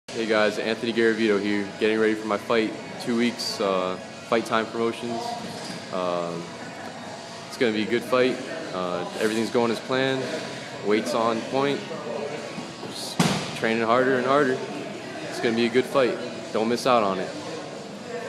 Anthony Garavito Camp Update #1
Anthony Garavito gives us a quick update on his camp at Evolution MMA for his Pro Debut fight June 17th vs. Leo Valdivia in FIGHT TIME 5